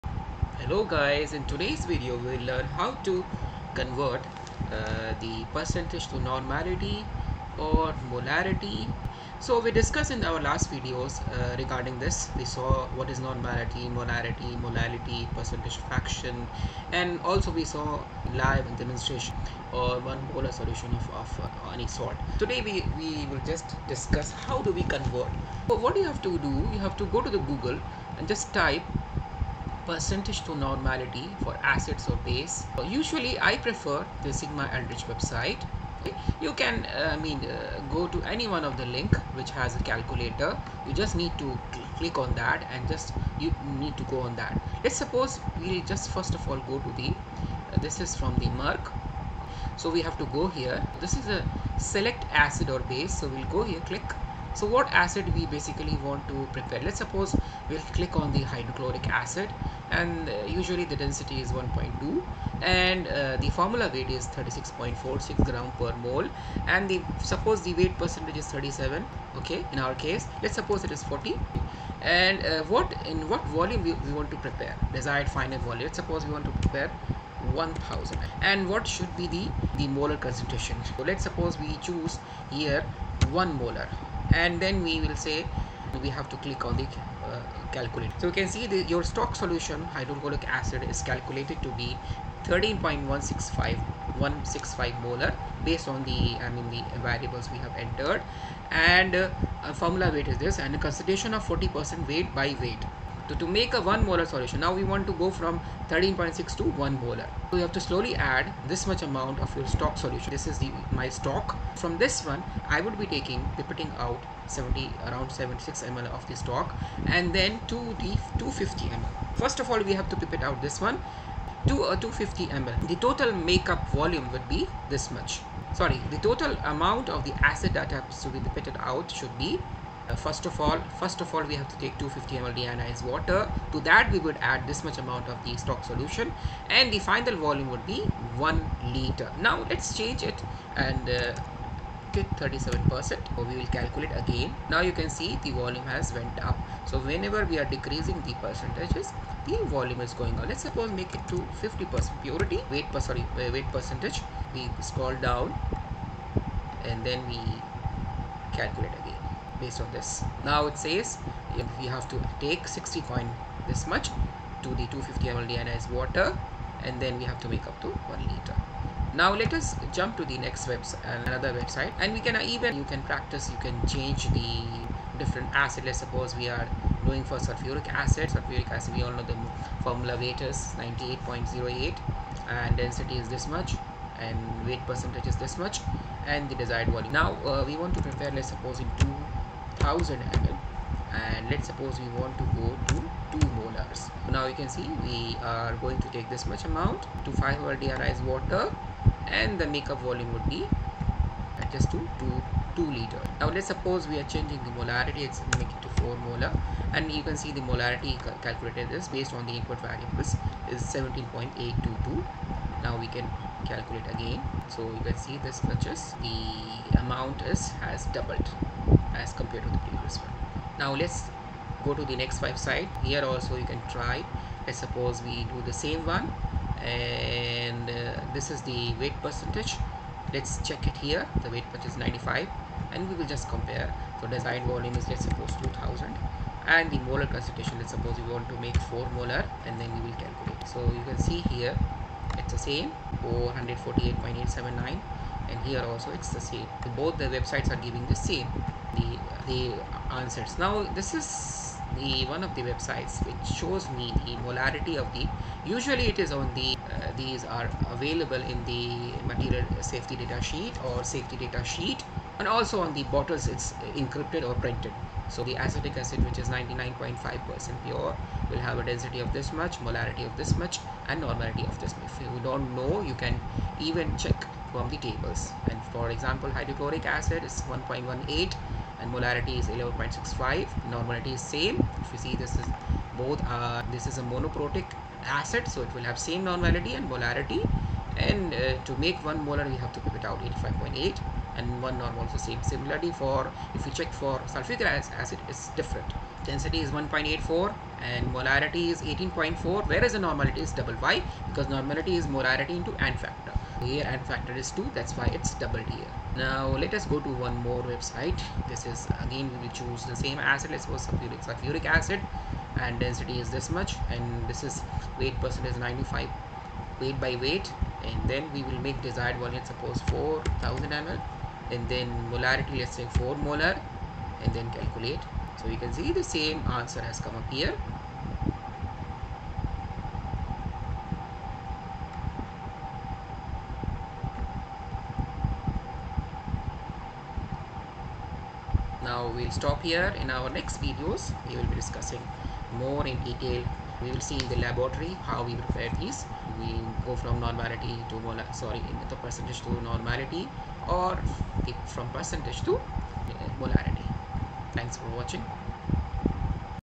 “How to Convert Percentage (%) to Molarity and Normality | Acids & Bases (Step-by-Step)”
In this video, I explain how to convert percentage (%) strength of acids and bases into Molarity (M) and Normality (N) using a simple, step-by-step method.
This concept is extremely important for laboratory work, environmental engineering, chemistry, and numerical problem-solving.
✔ What is percentage (%) strength of acids and bases
✔ Role of specific gravity
✔ Importance of purity (%) of chemicals
✔ Step-by-step conversion:
Percentage → grams per liter
Grams per liter → molarity
Molarity → normality
✔ Difference between Molarity and Normality
✔ Common examples:
HCl
H₂SO₄
NaOH
Other strong acids and bases
🧮 Key Formula Used
Molarity (M) = (Weight of solute in g/L) ÷ Molecular weight
Normality (N) = Molarity × n-factor
🌐 Website Used
I also demonstrate the calculation using an online calculation website, so students can easily verify their answers.
✅ Environmental Engineering students
✅ Chemical Engineering students
✅ Chemistry students
✅ Lab technicians
✅ Competitive exams & university exams